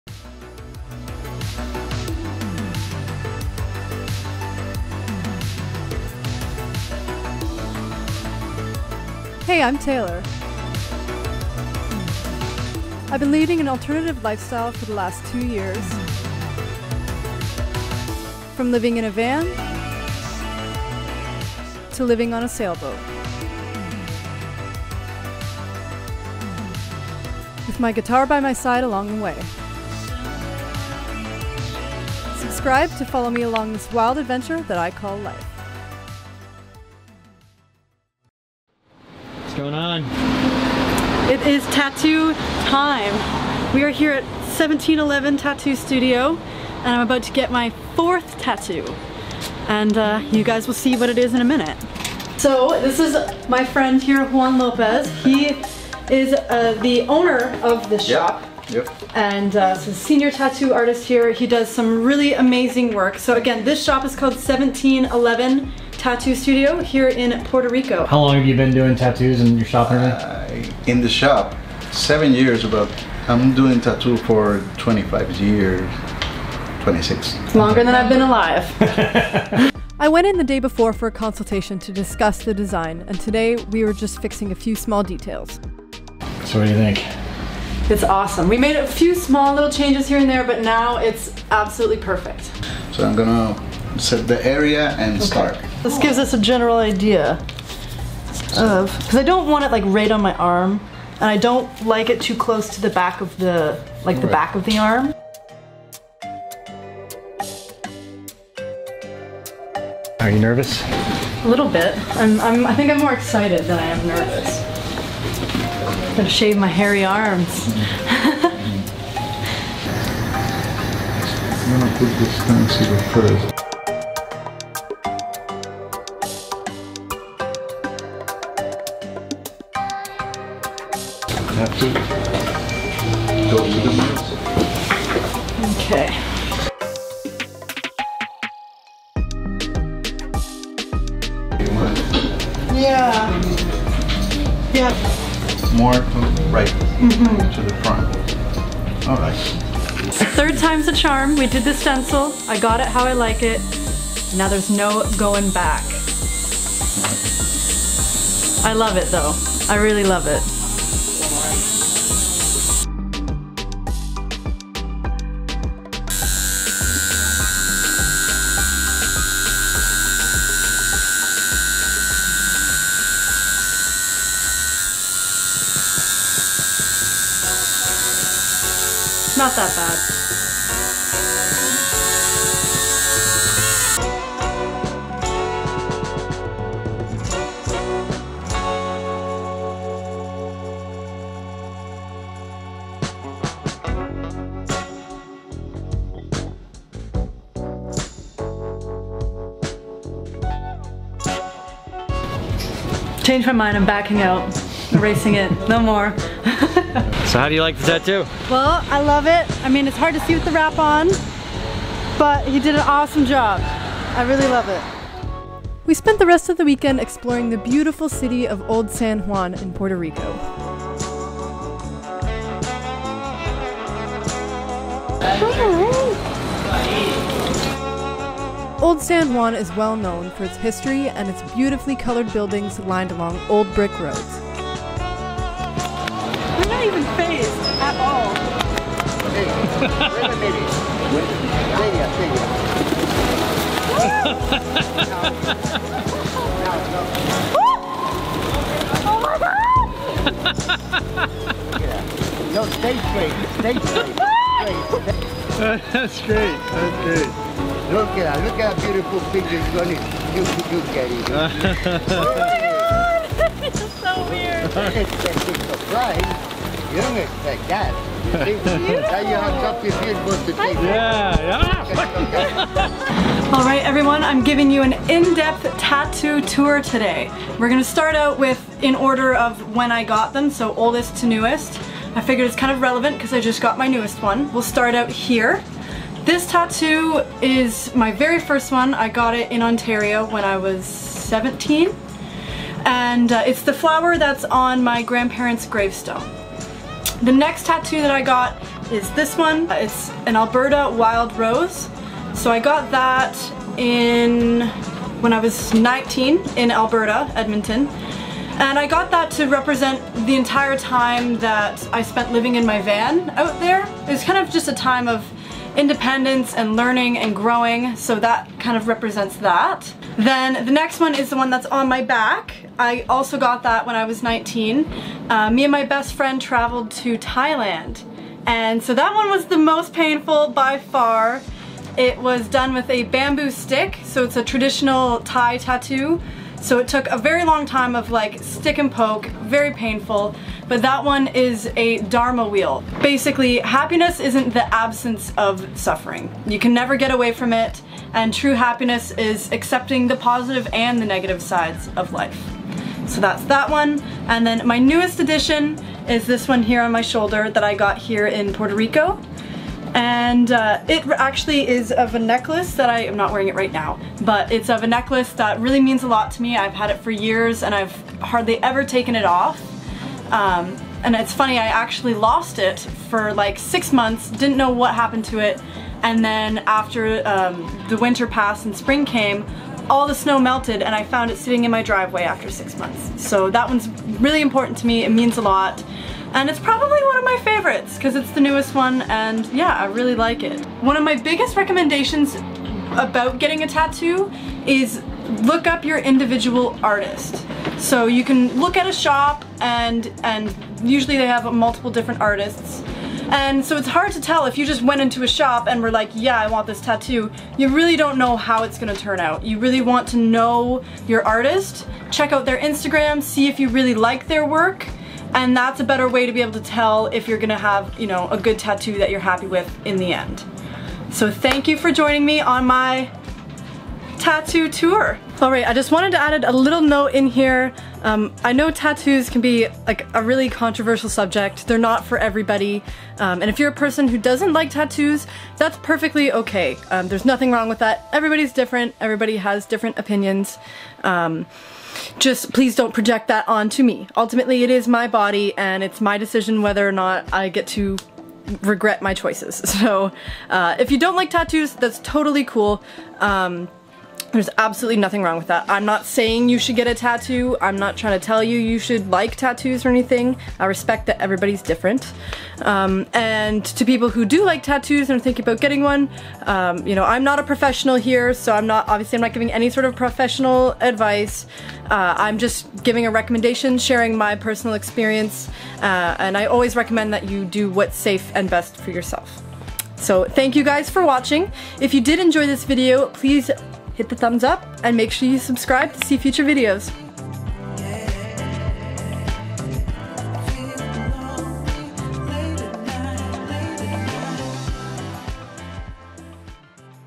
Getting a TATTOO in PUERTO RICO!
In this video I take you along with me as I get my 4th tattoo in Puerto Rico! I also go through and explain all of my tattoo's and their meanings. It was a very positive experience and I would highly recommend 1711 Tattoo Studio in San Juan, Puerto Rico.

Tattoo Shop Instagram: @1711tattoostudio
Tattoo Artist Instagram: @juanlopezart_